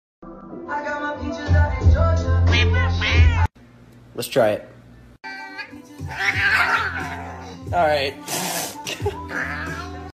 The second cat is possessed by a snake
Here is a funny video for you. The second cat is possessed by a snake, hahaha